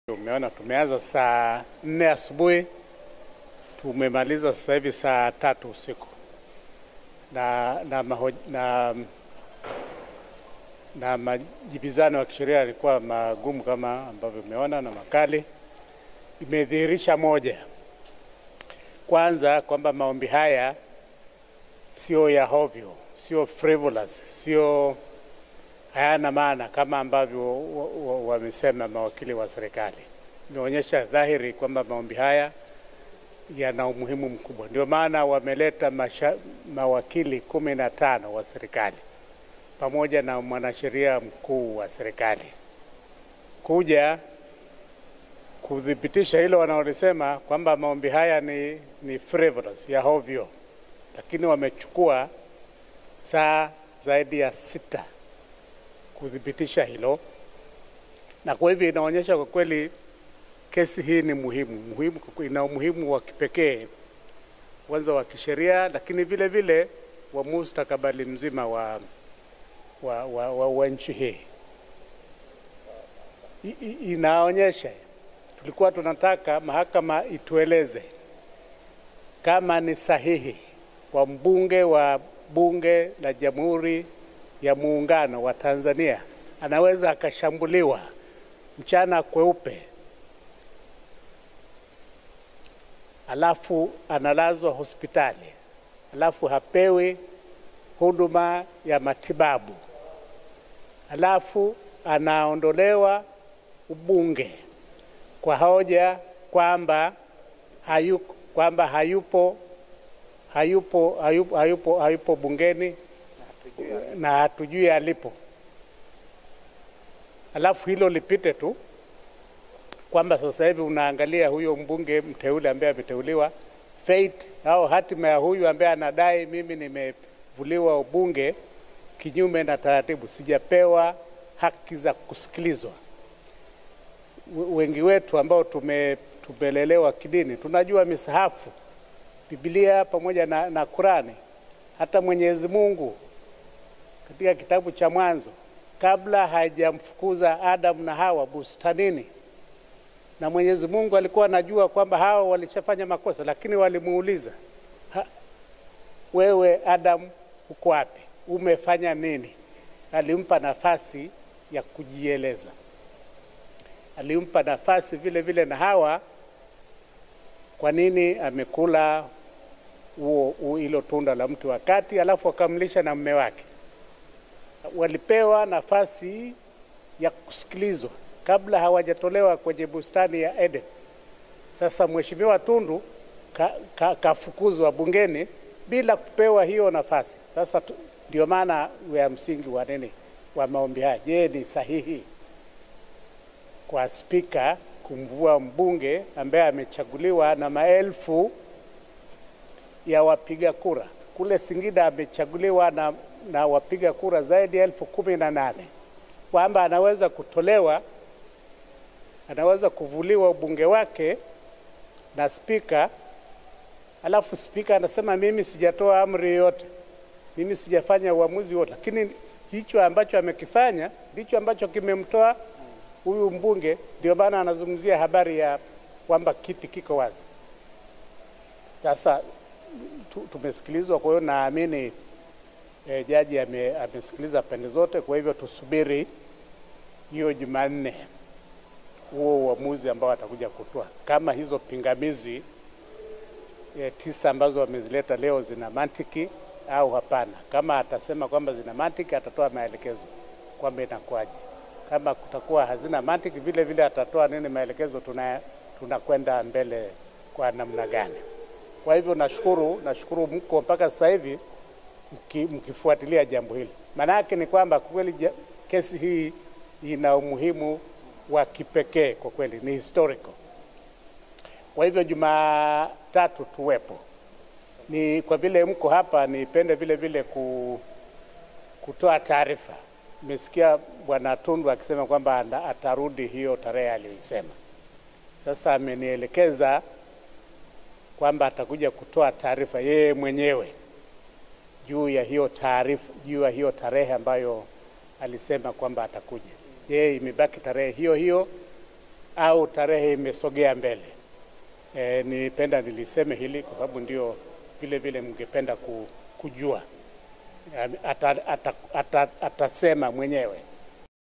Tundu Lissu aishangaa Serikali Wamsema kesi ni ya hovyo huku
wameleta mawakili 16 huku  wanasema kesi ni ya ovyo